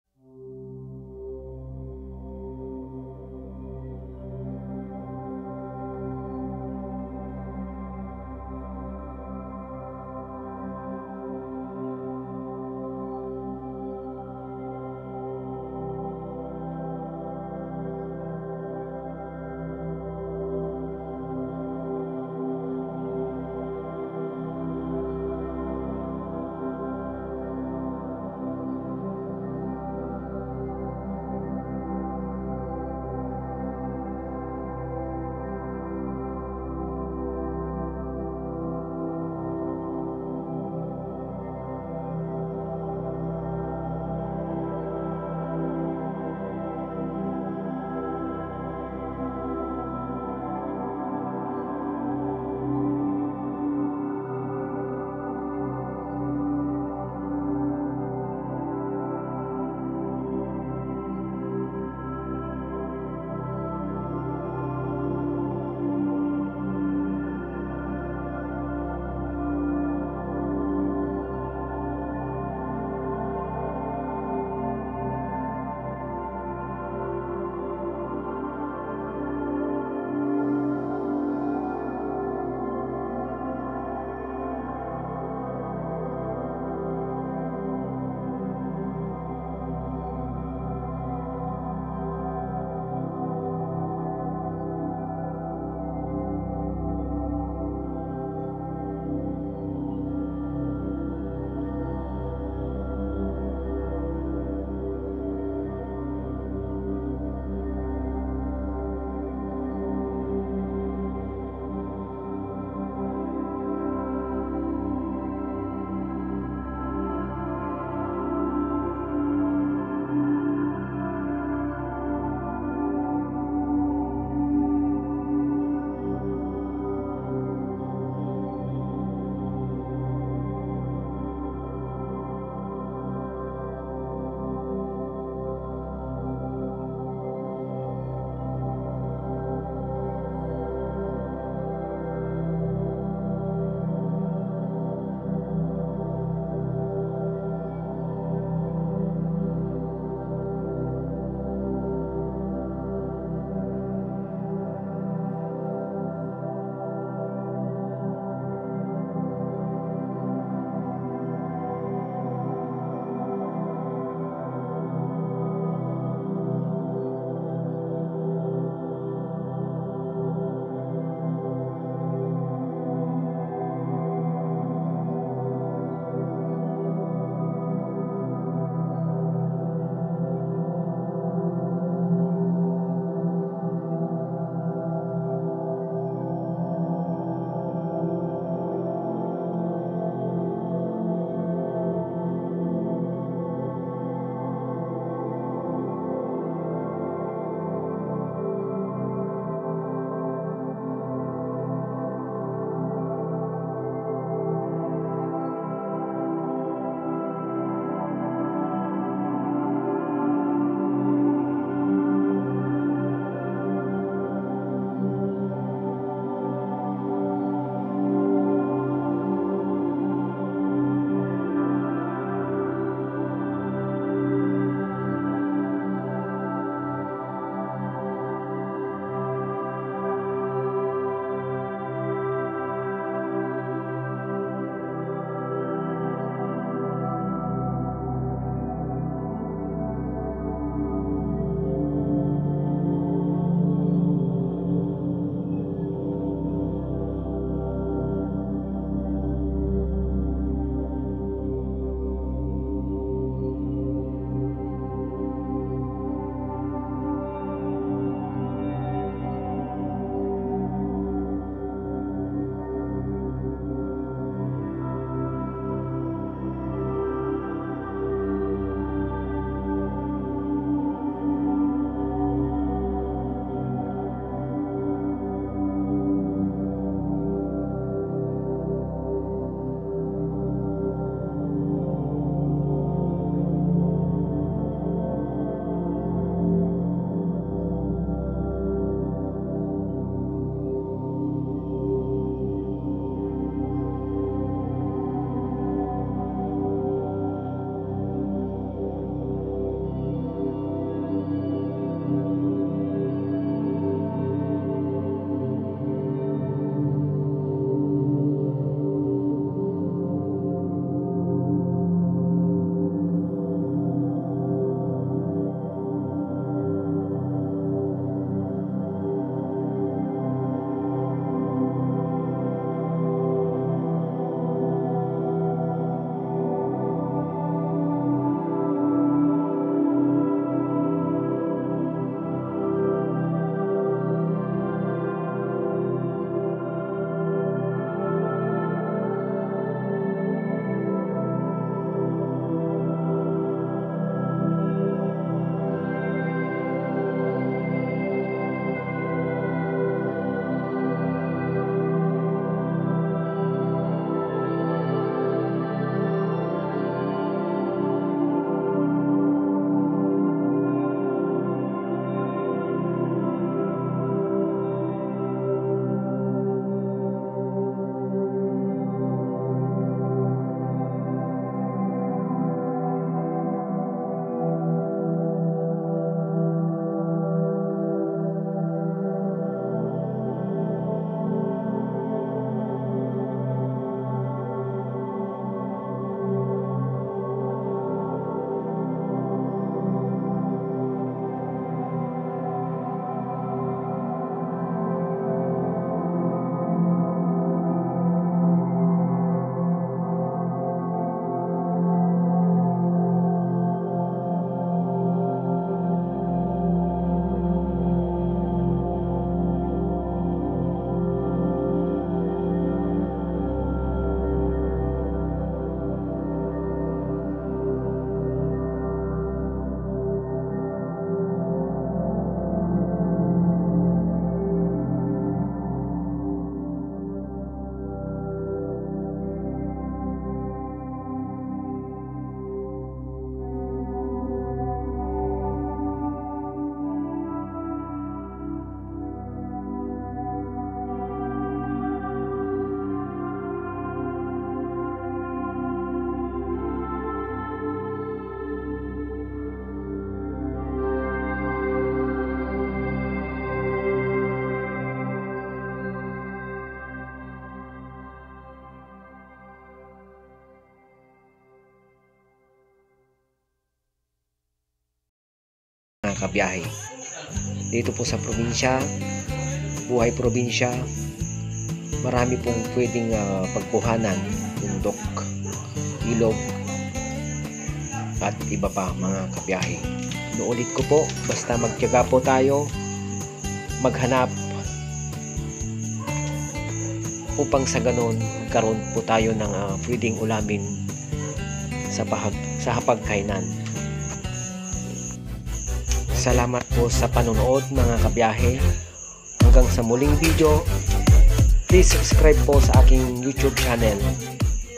Pagkuha ng Kulitis /kalunay, rabong/ labong at bagbagkong
Buhay probinsya wala ng hihigit pa ,
Preskong hangin ,berdeng kapaligiran ,
Maghanap ng ulam ang syang  isaalang alang
Ng  ang hapag kainan ay may pagsasaluhan..